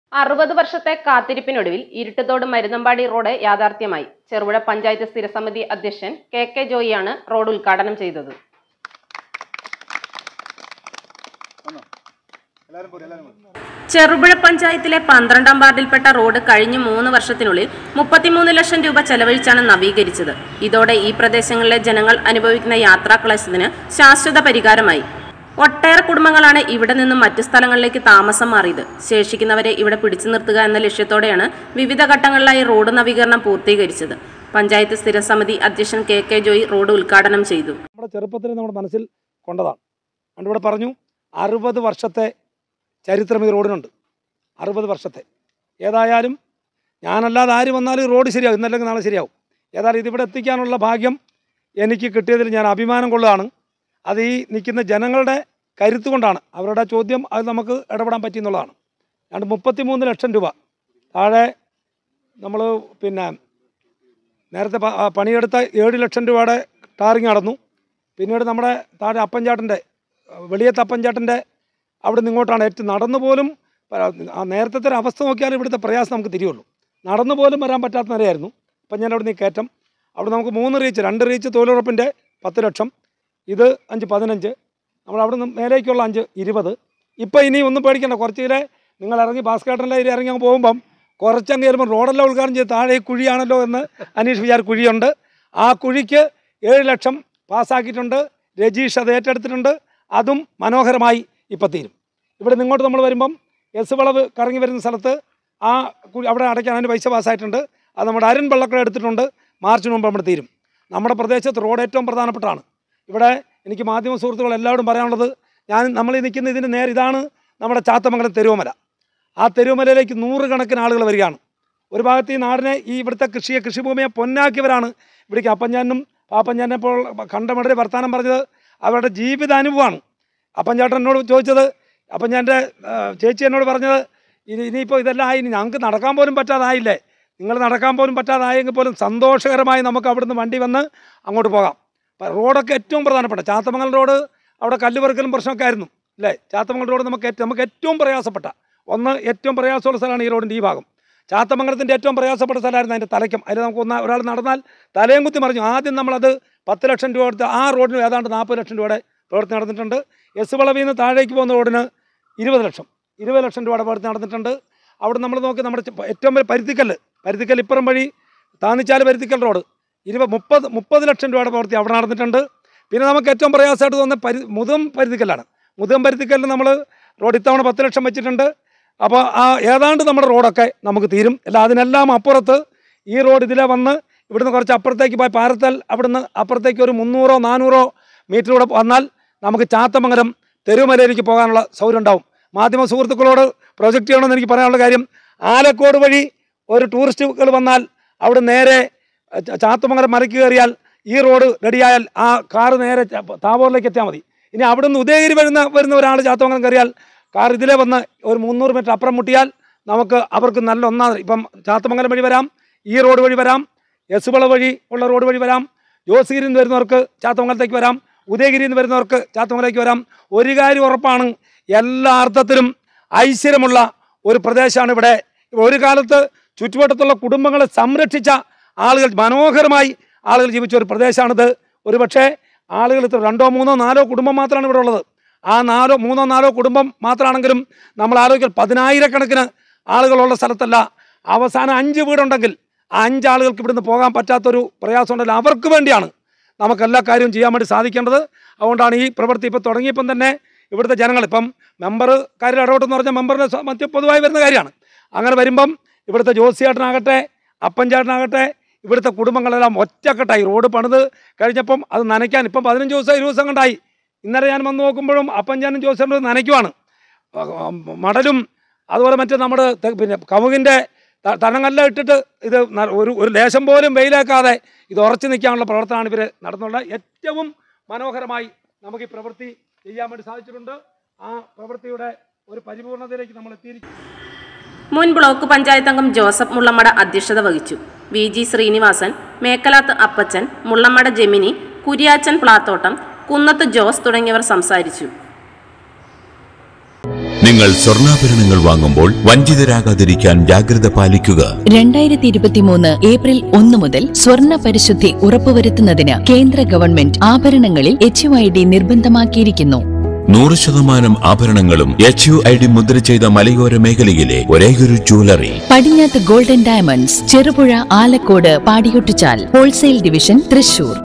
60 വർഷത്തെ കാത്തിരിപ്പിനൊടുവിൽ ഇരുട്ടുതോട് -മരുതുംപാടി റോഡ് യാഥാർഥ്യ മായി.
60 വർഷത്തെ കാത്തിരിപ്പിനൊടുവിൽ ഇരുട്ടുതോട് -മരുതുംപാടി റോഡ് യാഥാർഥ്യ മായി.